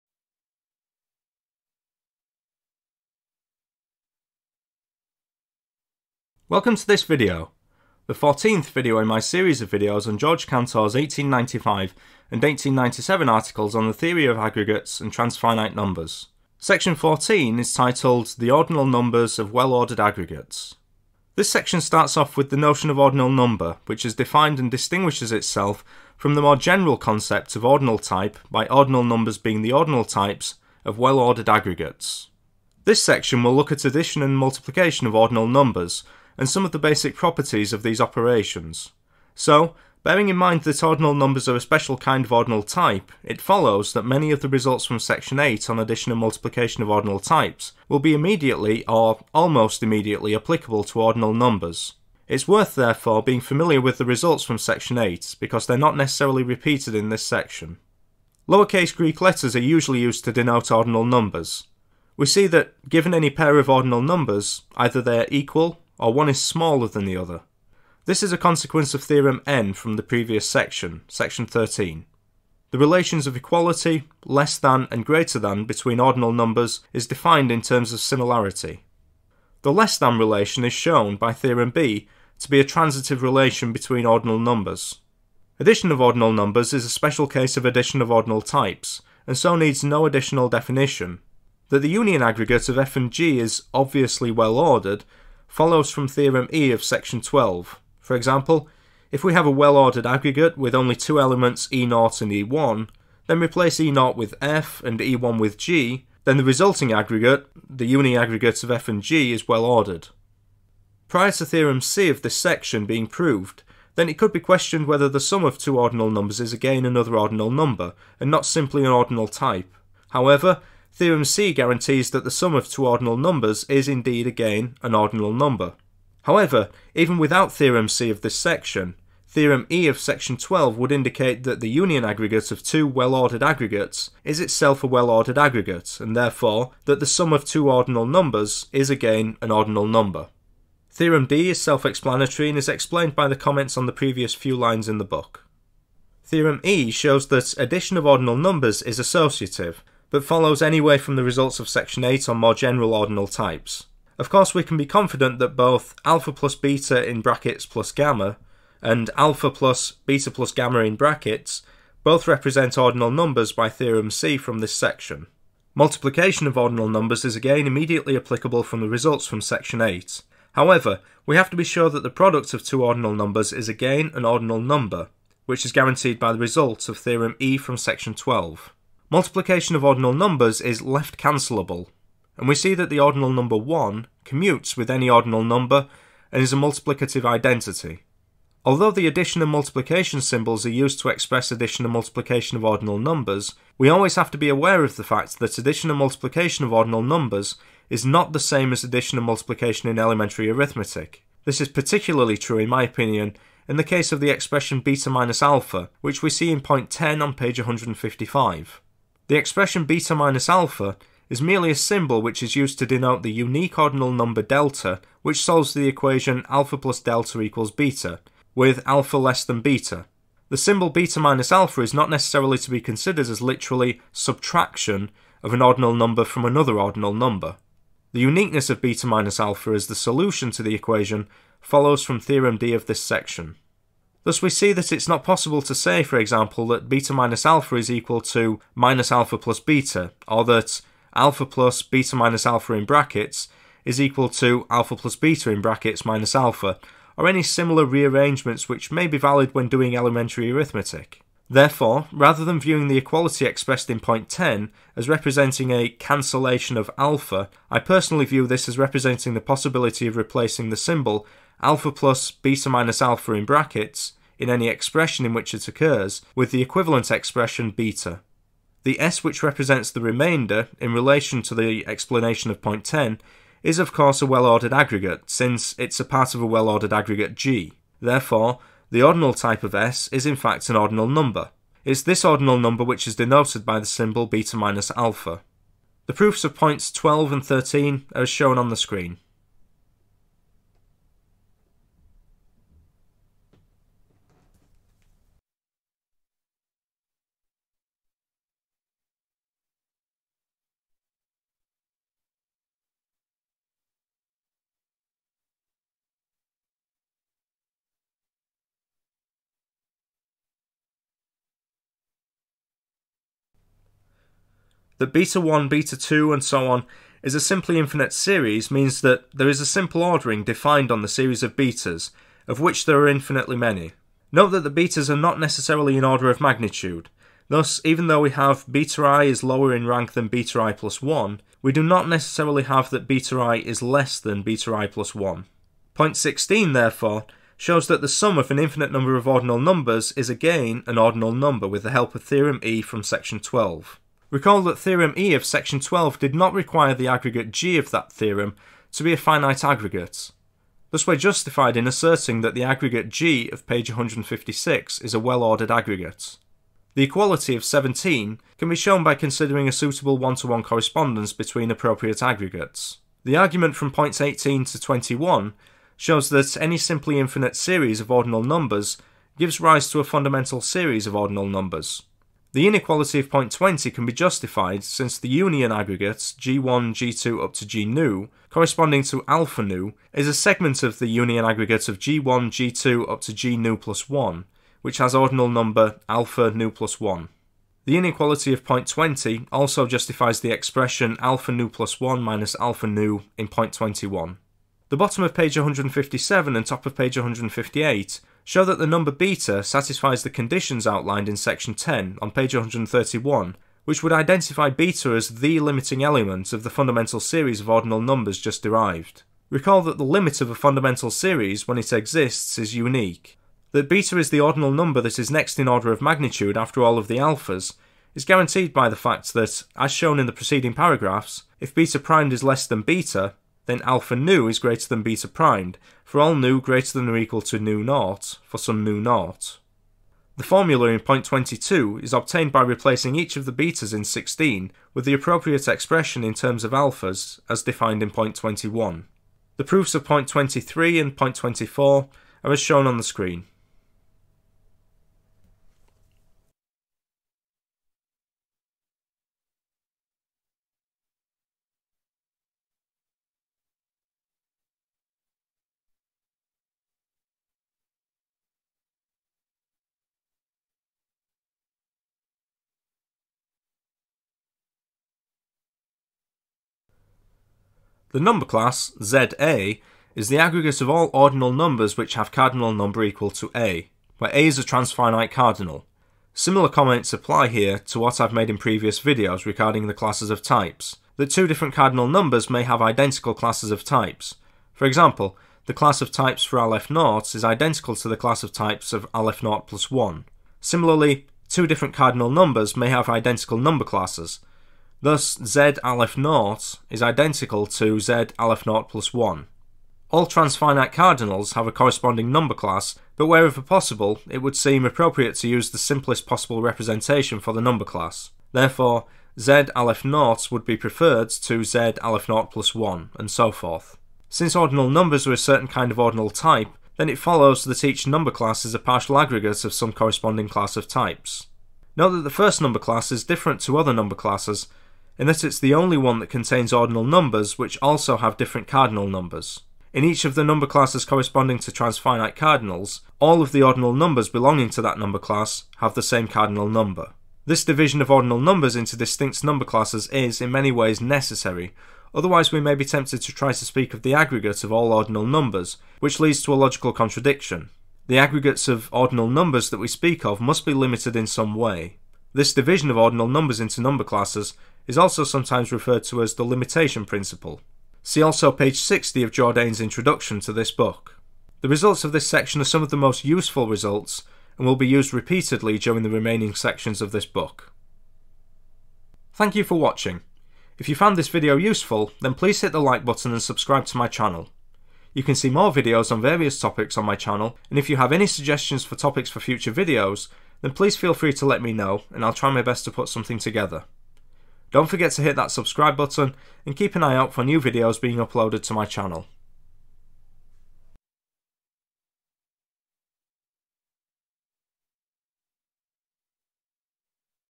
Georg Cantor - Contributions to the Founding of the Theory of Transfinite Numbers - Section 14
This video is video number fourteen in my series of videos covering the contents of the translations into English of Georg Cantor's famous 1895 and 1897 articles on the theory of aggregates and tranfinite numbers.

This video covers the material from section 14 of the 1897 article titled The Ordinal Numbers of Well-Ordered Aggregates. This section properly introduces the concept of ordinal number and goes on to discuss the basic operations of addition, subtraction and multiplication of ordinal numbers. Since ordinal numbers are a particular kind of ordinal type then the already given definitions of addition and multiplication of ordinal types carry straight over - however, there are, of course, certain properties which only apply to ordinal numbers which is what this section draws out. We also encounter the limit of a fundamental series of ordinal numbers and the definition of the second number class.

This is one of the most important sections going forward; the properties derived in this section are used repeatedly in later sections.

In this series of videos there will be a video for each section and the videos will be uploaded at a rate of one or two per week (with maybe a few weeks break between the two articles).